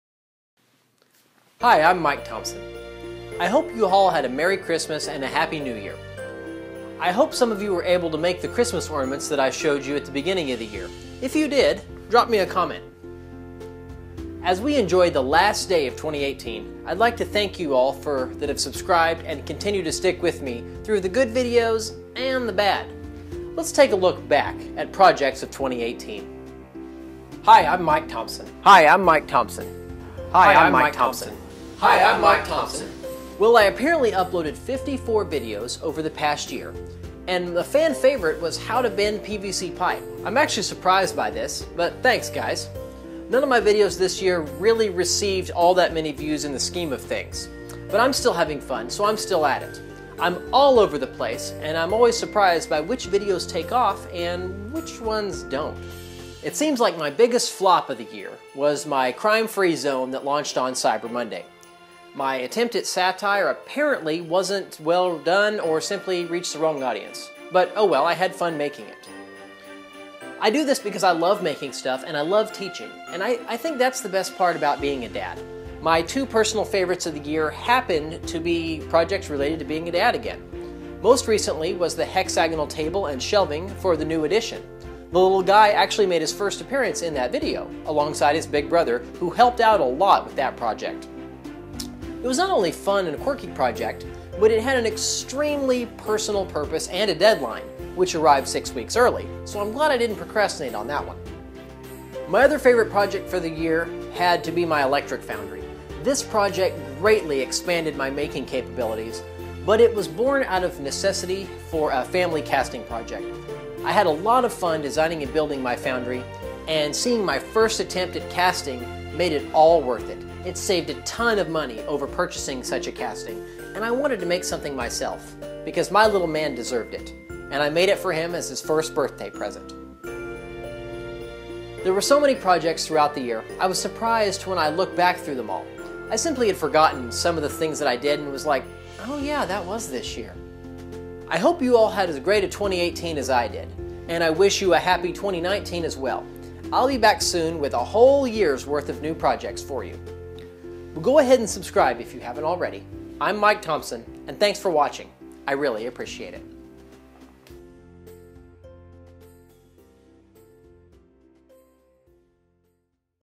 2018 ReCap & Thanks- Mikes Inventions
The title of the video basically says it all. I certainly couldn't feature bits of all the projects this year, but I highlight some of my favorites and at least list all the titles that I uploaded in 2018. It's more like a notification.Oh well, too late now YouTube.